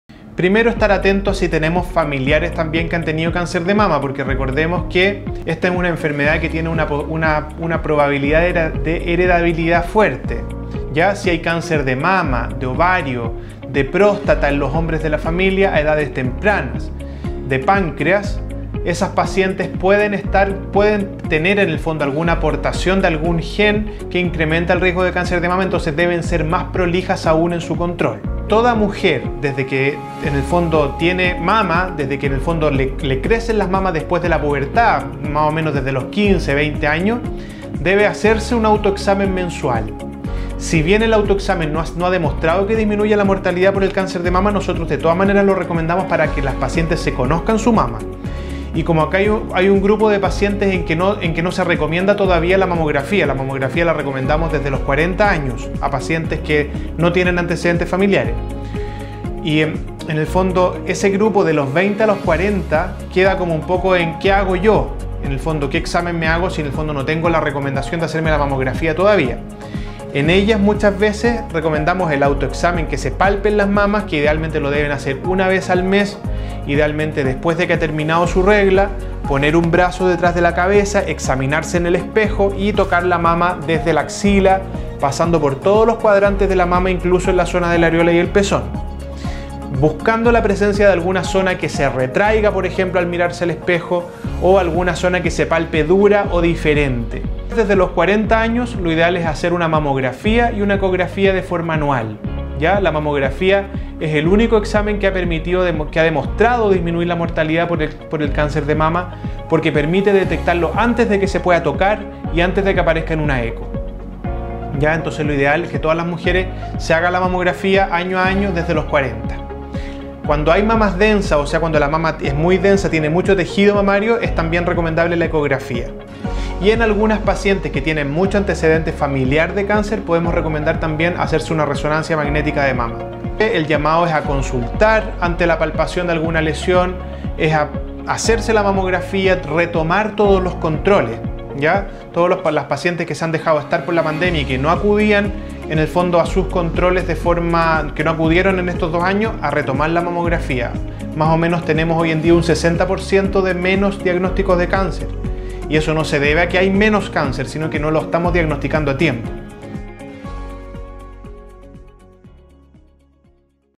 Dr. Badir Chahuán aconseja acerca de prevención cáncer de mama
Conversamos con el 𝗗𝗿. 𝗕𝗮𝗱𝗶𝗿 𝗖𝗵𝗮𝗵𝘂𝗮́𝗻 🩺, especialista en Cirugía Oncológica y Reconstructiva de Mama, de la Fundación Arturo López Pérez, quien aconseja acerca de la prevención del cáncer de mama, una de las principales causas de muerte en mujeres de nuestro país.⁣